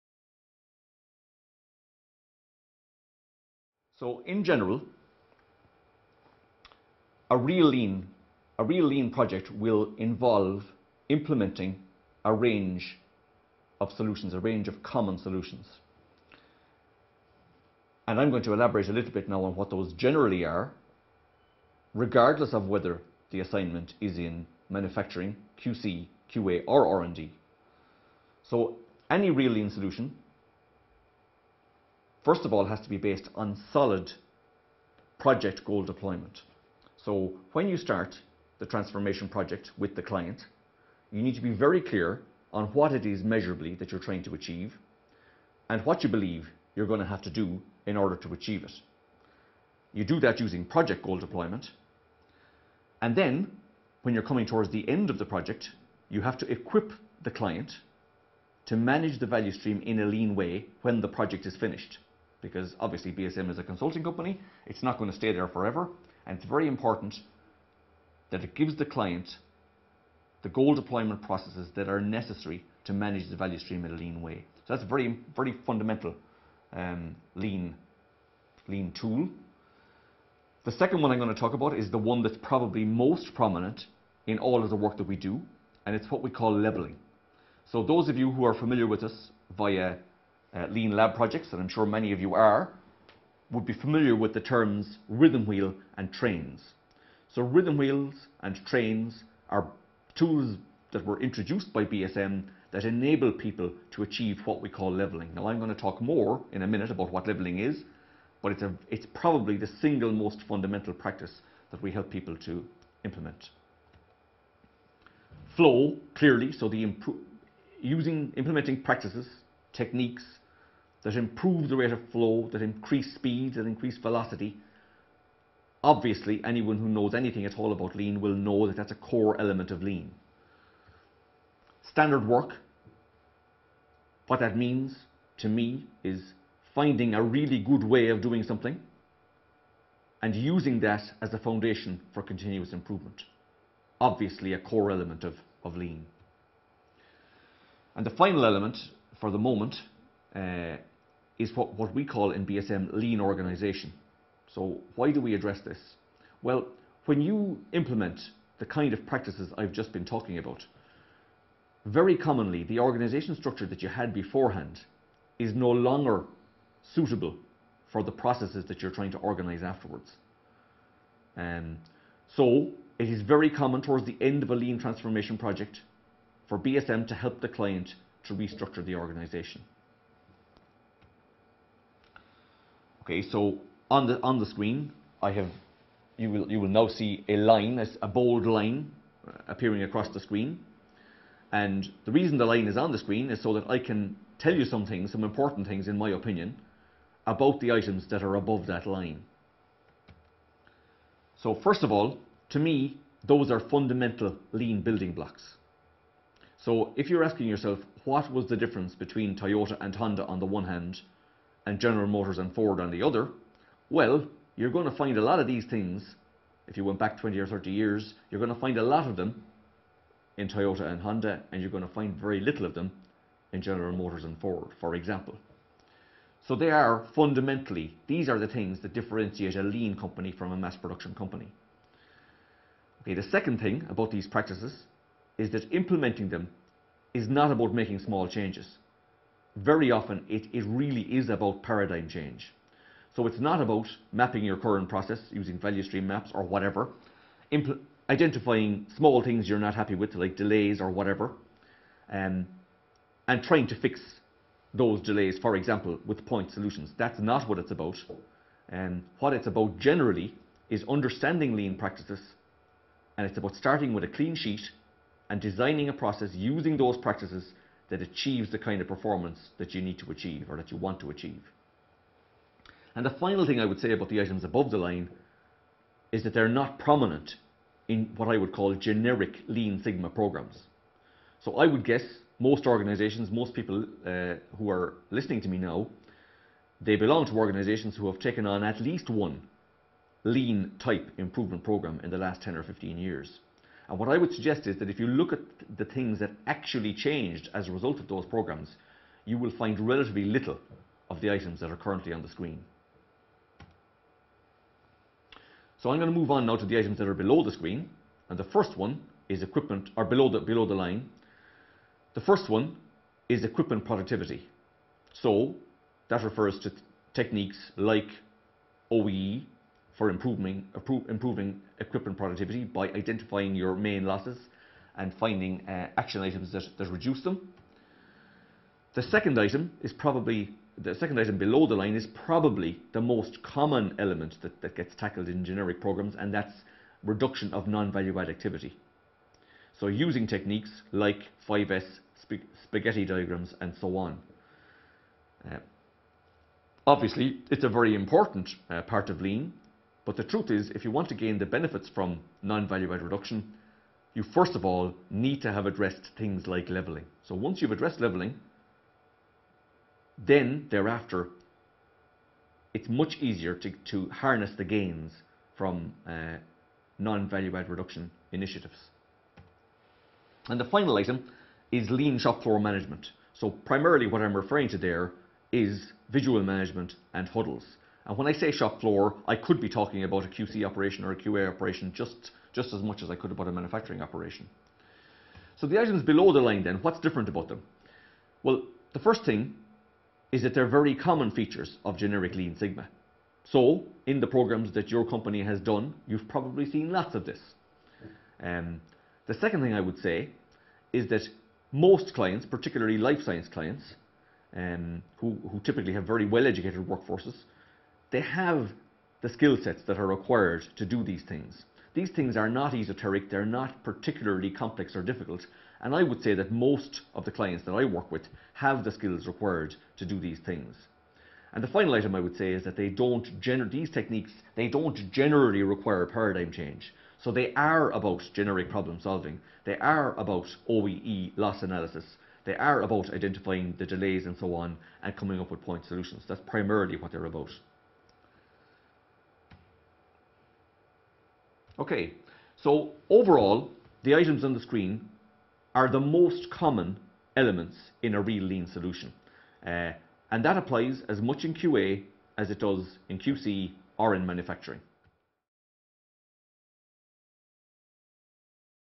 The Most Common Interlinked Elements in Real Lean Solutions
In this video highlight Maurice Hannon, Managing Director BSM (Lean and Operational Excellence Consultants) talks about Real Lean in QA. Here he addresses 'The Most Common Interlinked Elements in Real Lean Solutions'. For further information and videos check out